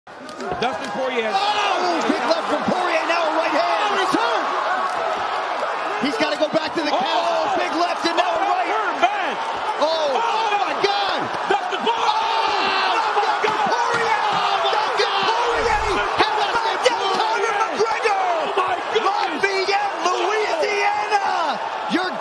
Бой Порье с Конором
Как это было 🔥

Просто сокрушительные удары Дастина